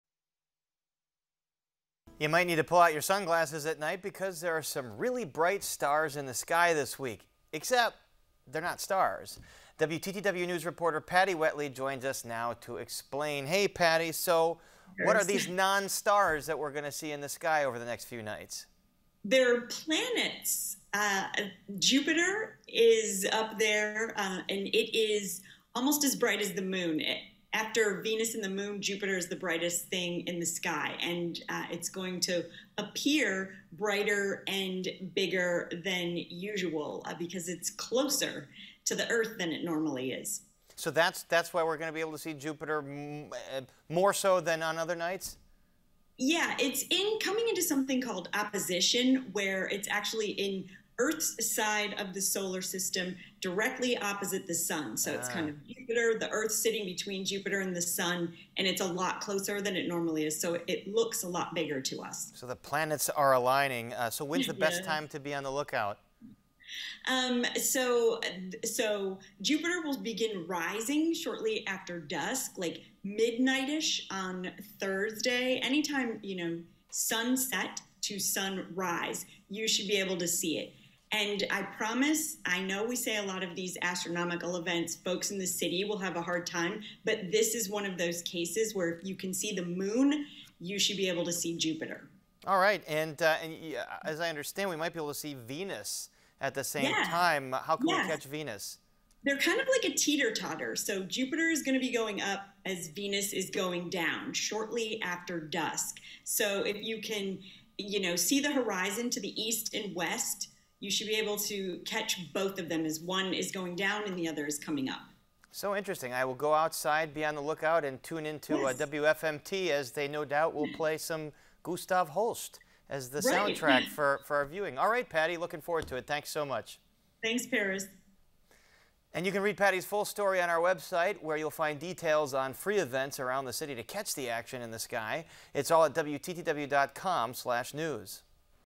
Jupiter Is at Its Biggest and Brightest Right Now
You won’t just be seeing stars in the sky this week. Jupiter is now about as close as it ever gets to Earth. WTTW News reporter Patty Wetli explains why.